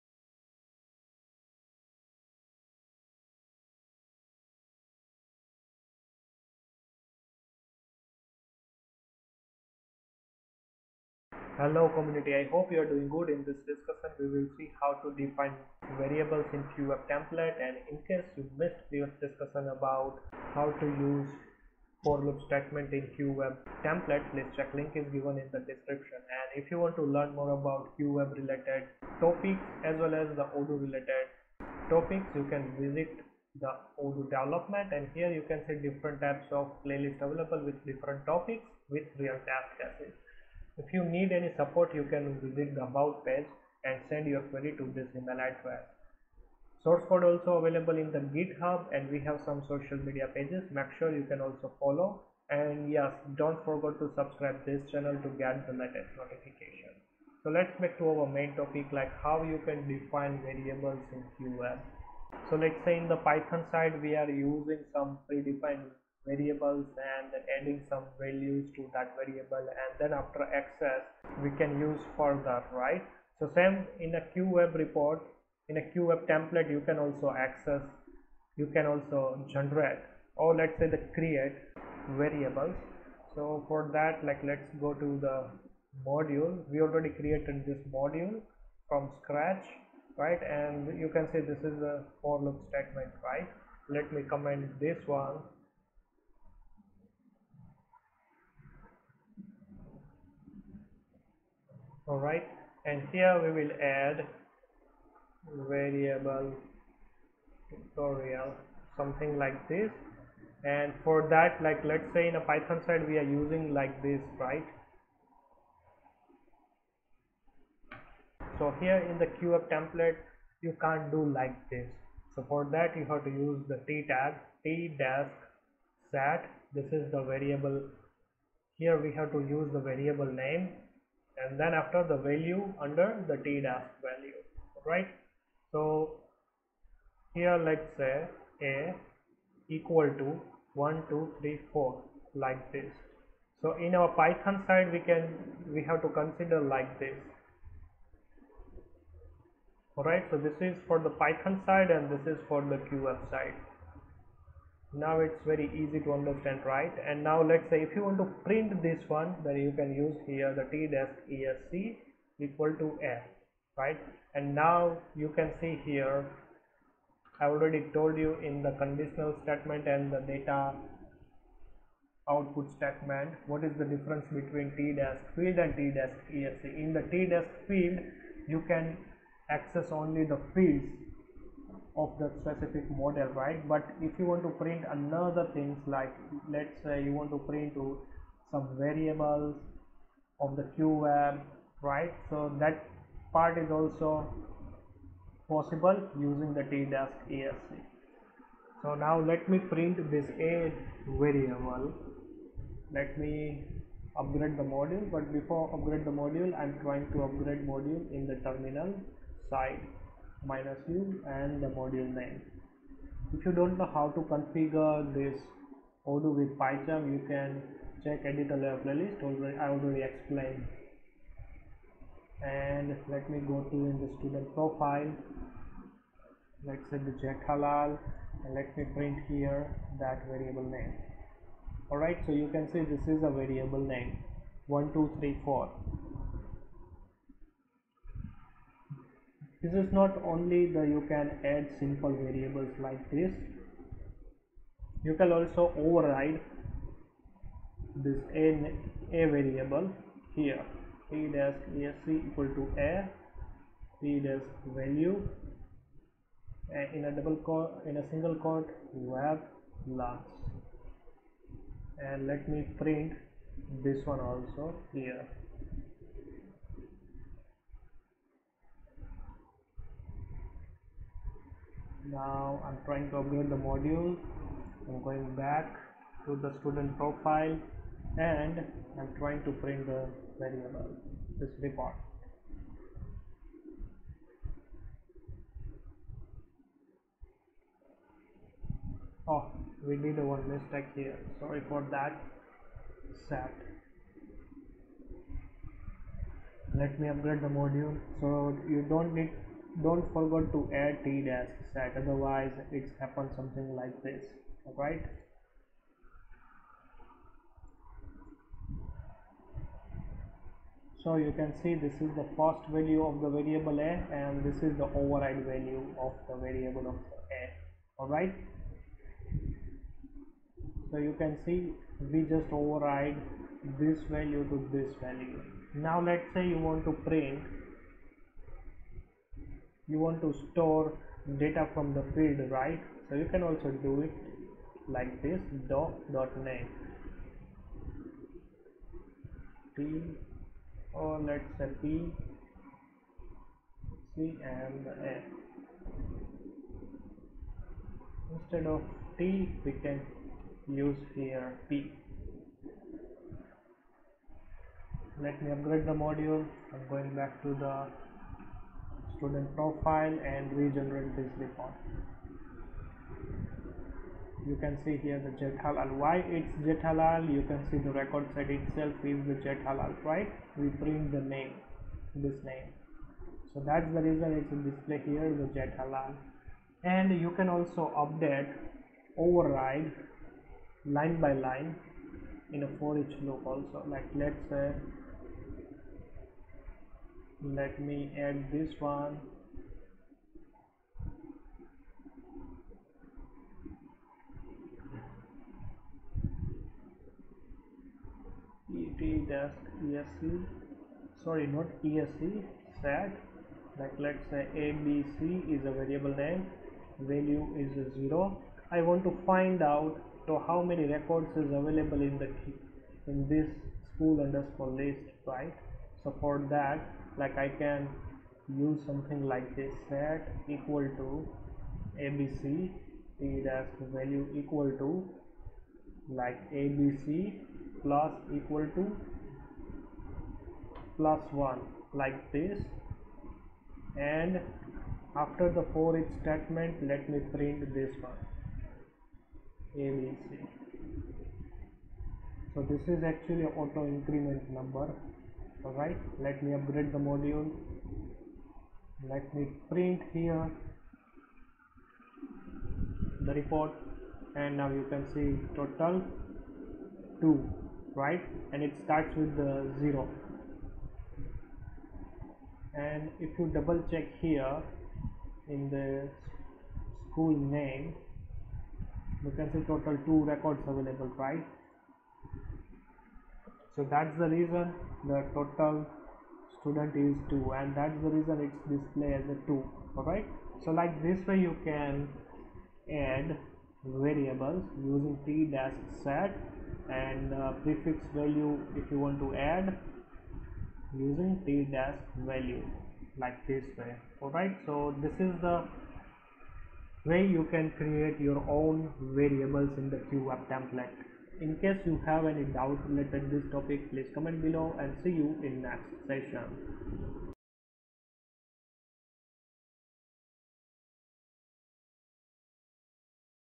How to create variables in Qweb pdf template Odoo | Define variables
How to create variables in qweb template odoo, t-set t-value tags to generate variable in qweb template. define your own custom variables and you can override those variables. Please check till end of this discussion.

In case you missed previous discussion about How to use pre define variables in looping statement. Please check below link.

Support me :-